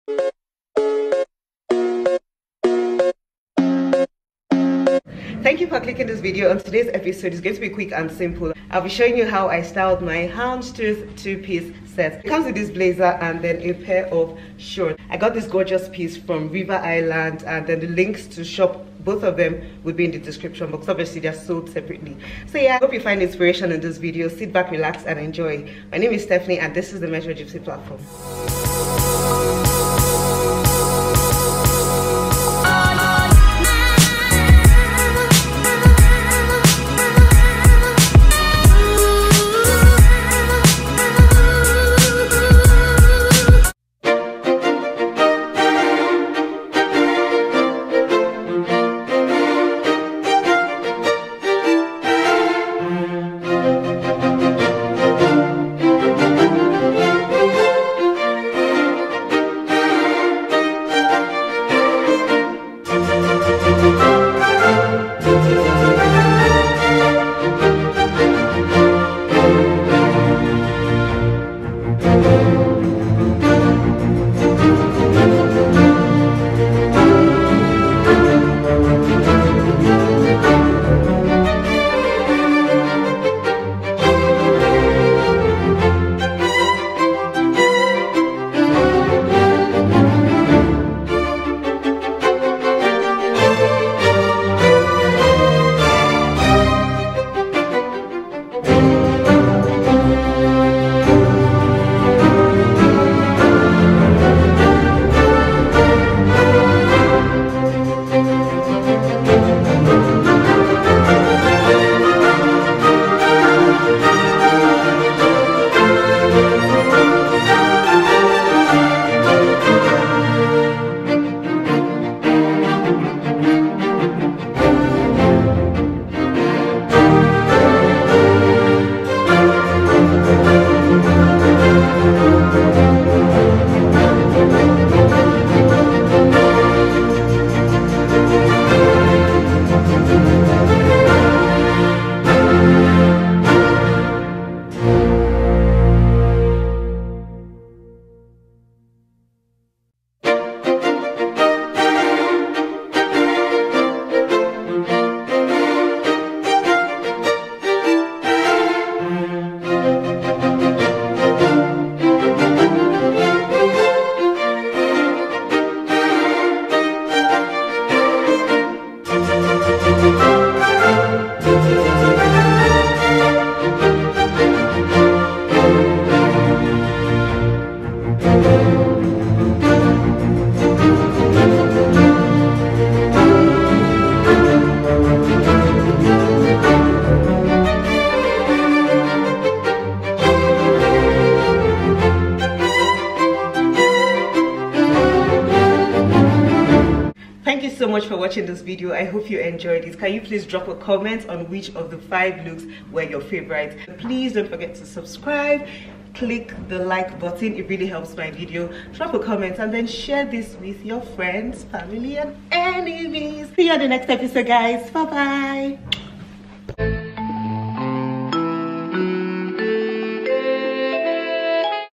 HOW TO STYLE: GINGHAM FOR SUMMER 2021 ImWearingRI | metrogypsie
✅ wearing suits in print fabric gets a bit tricky for some. Copped this beautiful suit from River Island and decided to share 5 ways to wear it.

TIMESTAMP
N/A